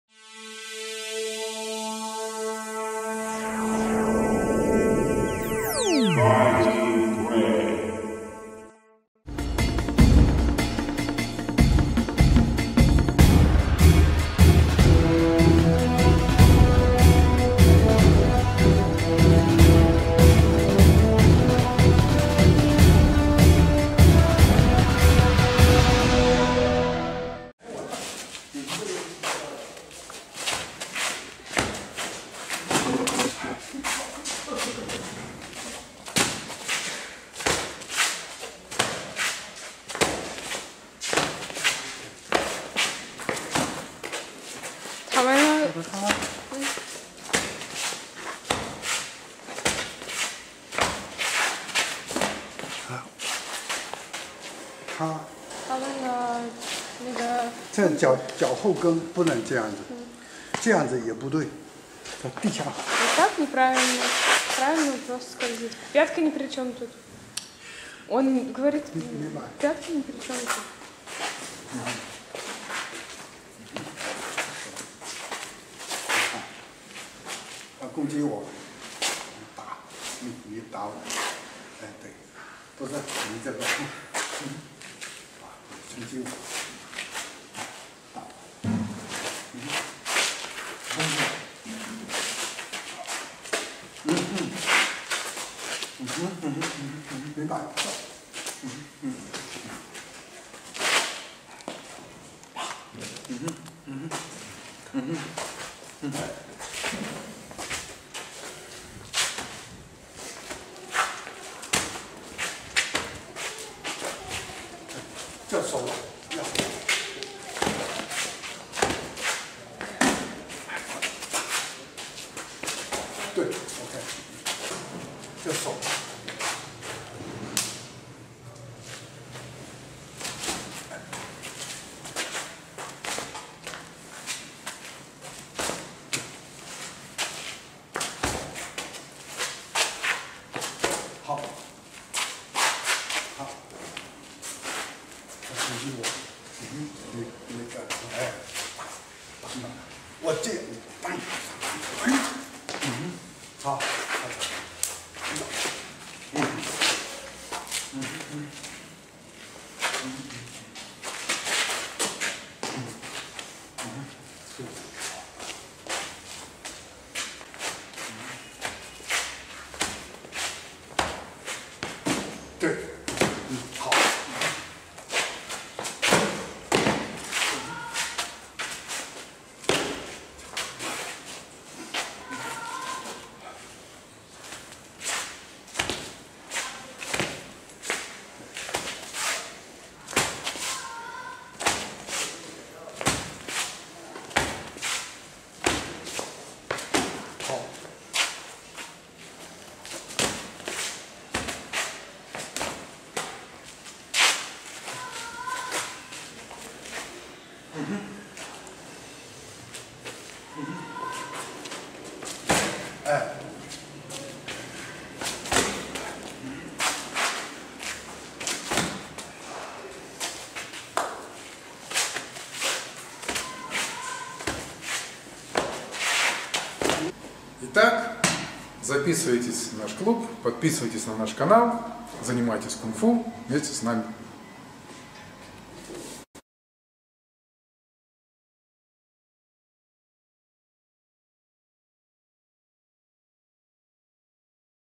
Minghe-quan training notes: knees, kicks and distance
Minghe The Calling Crane has its own characteristics in the performance of technical elements, that are closely related to the tactics of combat.
This is not only about hand techniques, but also about kicking.

Here, Yu Shifu explains to me that there is no need to turn the heel forward during the kick, as I am used to, practicing Taekwondo. In Minghe, the distance is much closer - and despite the fact that this focus makes the kick stronger, the support leg becomes easier to sweep out